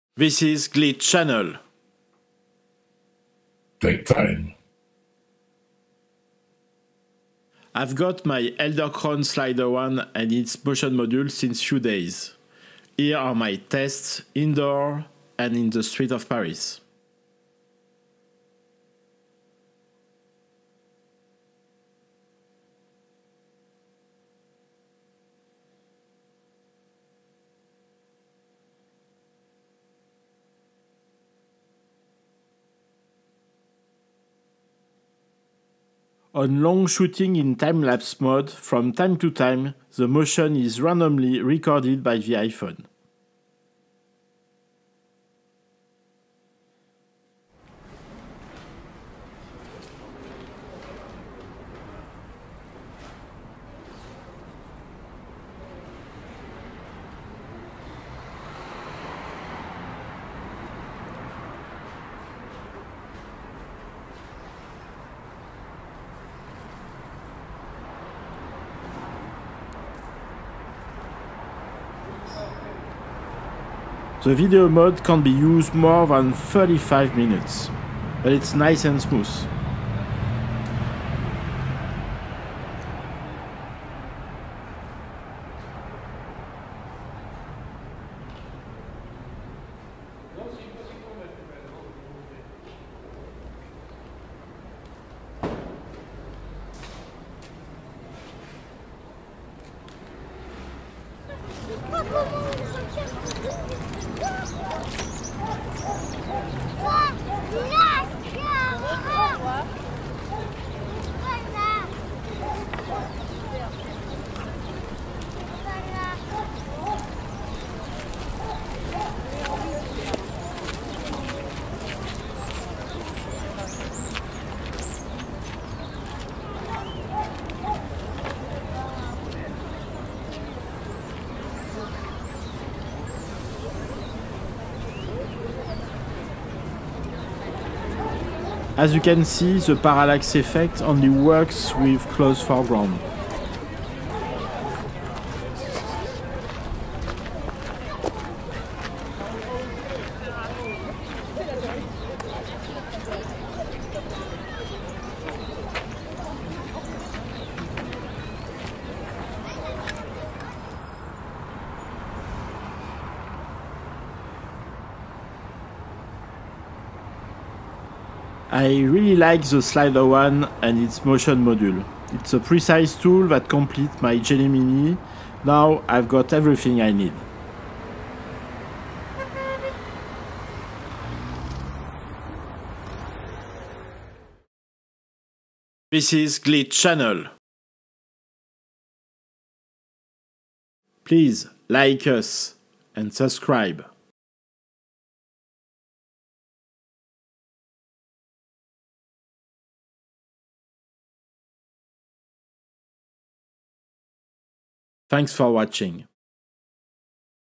Timelapse + iPhone 7 Plus & the Edelkrone SliderOne / Review / Paris / 4K
Here is my review after few days with the Edelkren SliderOne Motion Module. Timelapse were recorded with my iPhone 7 Plus and the Application ProCam.
The iPone head is a MePhoto Sidekick mph200.
That beautiful Square is the "Square Rapp".
Question for a Champion :
From what movie the cat is from ?